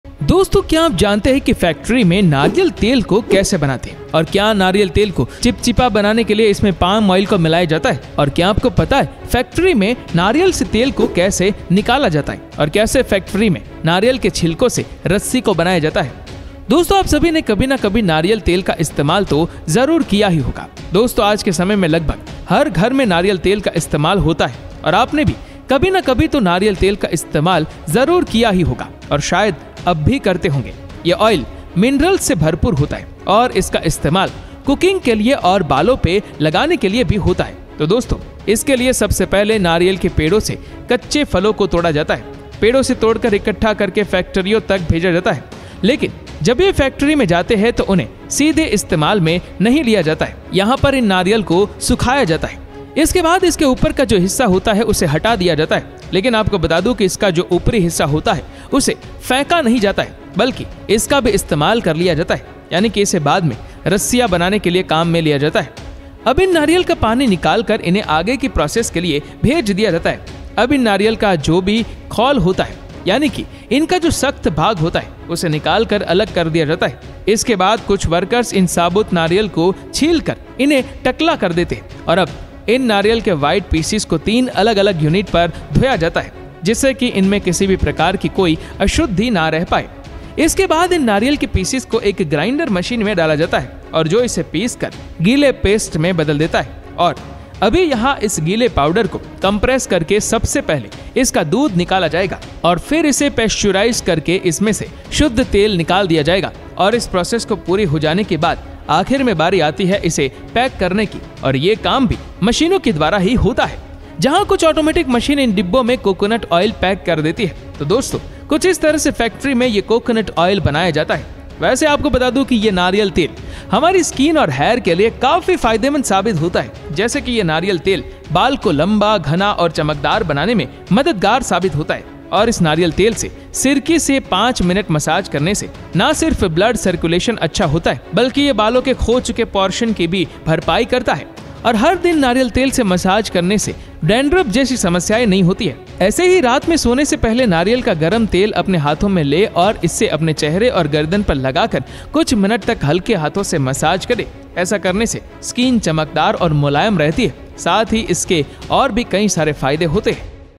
फैक्ट्री में नारियल तेल कैसे बनता है? | Coconut Oil Manufacturing - How it's Made | The Fact Zone
नारियल तेल Factory में ऐसे बनाया जाता है | Coconut Oil Manufacturing - How it's Made

        Friends, do you know how coconut oil is made in a factory?, and is palm oil mixed in coconut oil to make it sticky?, and do you know how oil is extracted from coconut in a factory? And how is rope made from coconut shells in a factory?
Friends, all of you must have used coconut oil at some time or the other. Friends, today coconut oil is used in almost every house and you too must have used coconut oil at some time or the other. And probably still do.
This oil is rich in minerals and is used for cooking and also for applying on hair. So friends, for this, first of all raw fruits are plucked from coconut trees. It is plucked from trees, collected and sent to factories. But when they go to the factory, they are not used directly. These coconuts are dried here.
After this the part above it is removed. But let me tell you that its upper part does not want to be thrown away, rather it is also used. That means it is later used to make Rasiya. Now the water from these coconuts is extracted and sent for further processing.
Now, these coconuts have their shell, that is, their hard part. It is taken out and set aside. After this, some works in South peel the coconuts and grind them. And now these coconut white pieces are washed on three different units so that no impurities of any kind remain in them.
After this, these coconut pieces are put in a grinder machine which grinds it and turns it into a wet paste and now this wet powder. First the milk will be extracted by compressing it and then by perforating it pure oil will be removed from it. And after this processor is completed, finally the turn comes to pack it and this work is also done by machines only.
While some automatic machines pack coconut oil in these containers, friends, this is how coconut oil is made in the factory. Well, let me tell you that this coconut oil proves to be very beneficial for our skin and hair. Like this coconut oil proves helpful in making the hair long, thick and shiny and massaging with this coconut oil mixed with vinegar for 5 minutes not only improves blood circulation but it also compensates the lost portion of the hair.
And massaging with coconut oil every day does not cause random problems. Similarly, before sleeping at night, mix warm coconut oil in your hands and apply it on your face and neck and massage gently for a few minutes. By doing this the skin remains shiny and soft. It also has many other benefits.